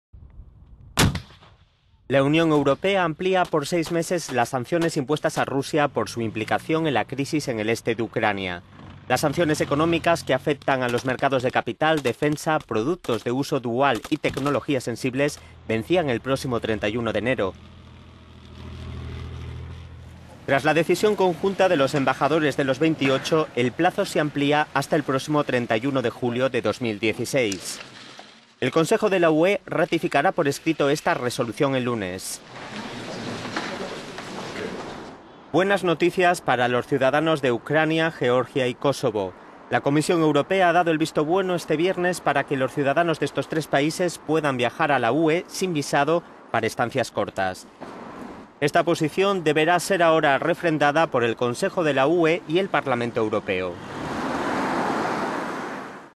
Bruselas amplía las sanciones a Rusia. Vía libre a Ucrania, Georgia y Kosovo para viajar sin…
La Unión Europea amplía por seis meses las sanciones impuestas a Rusia por su implicación en la crisis en el este de Ucrania

Las sanciones económicas que afectan a los mercados de capital, defensa, productos de uso dual y tecnologías sensibles, vencían el próximo 31 de enero.
Tras la decisión conjunta de los embajadores de los Veintiocho, el plazo se amplía hasta el próximo 31 de julio de 2016. El Consejo de la UE ratificará por escrito esta resolución el lunes.